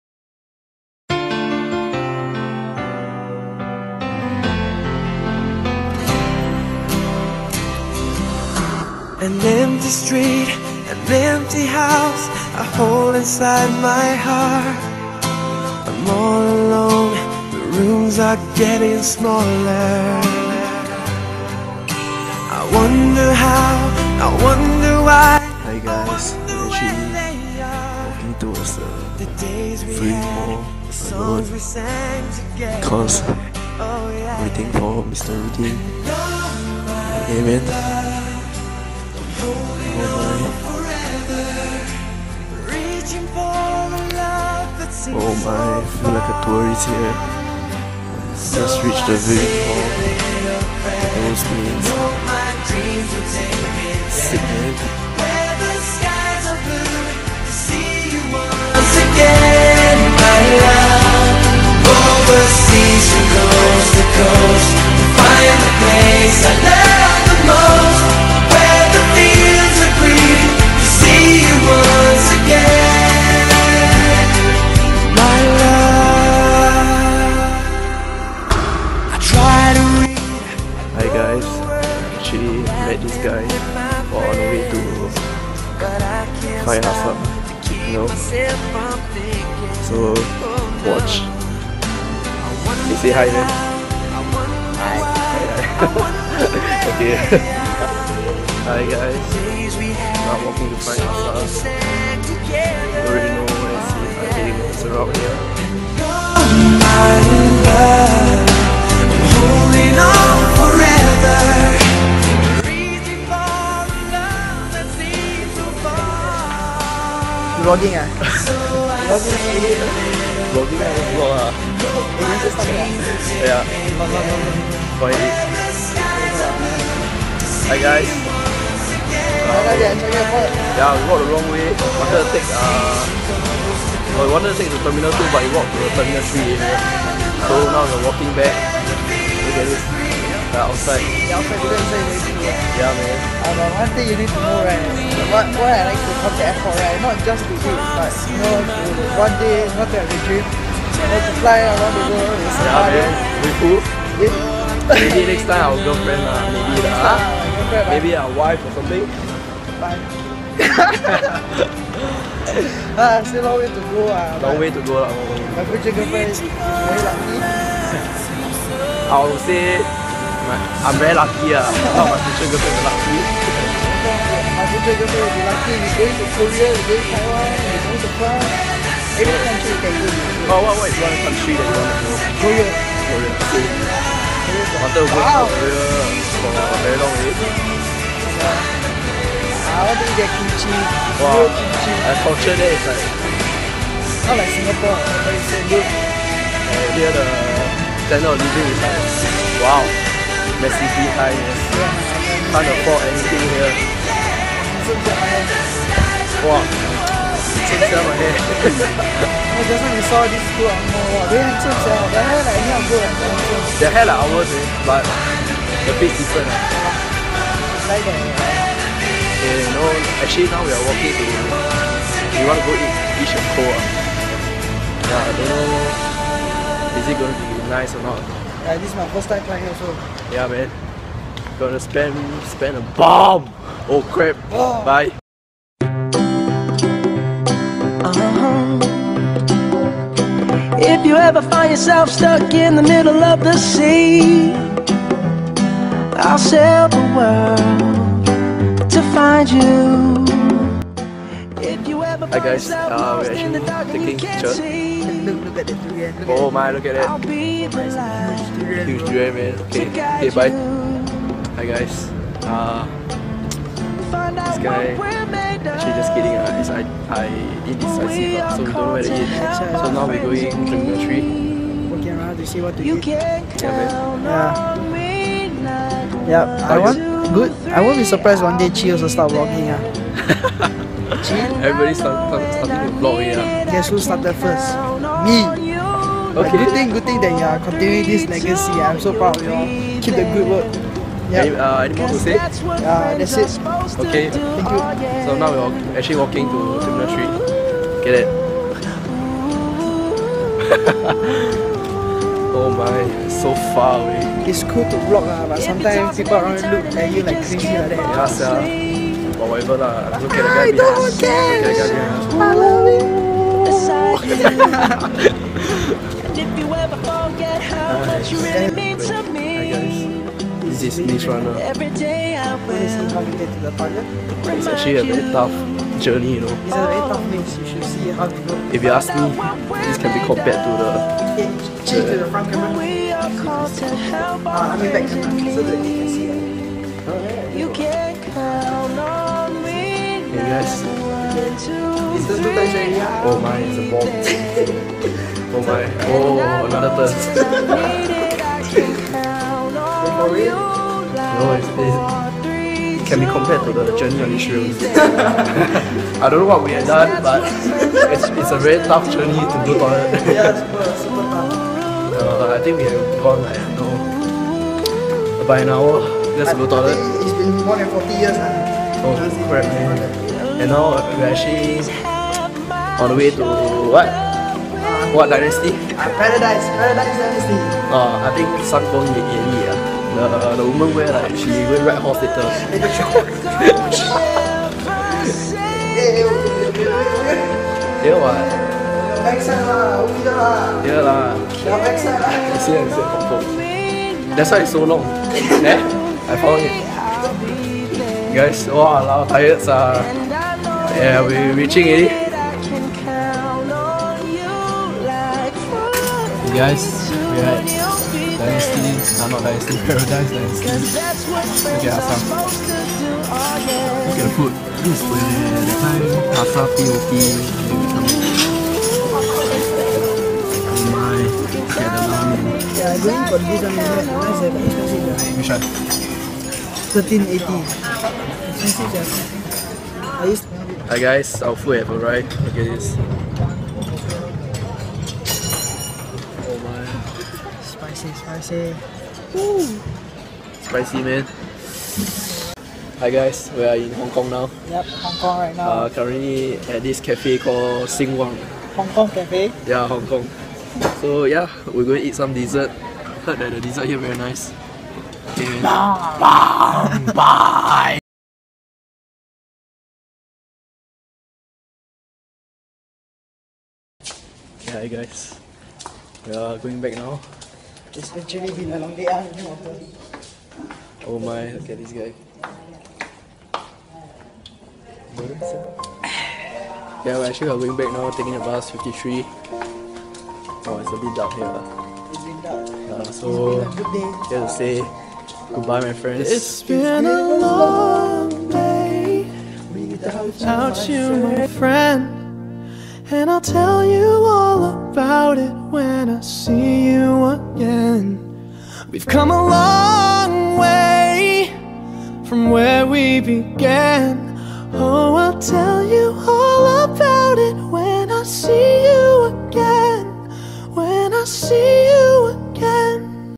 A day at Changi Airport with Rudding
A fun day with rudding. meeting after 10 thousand years, its been a long day~~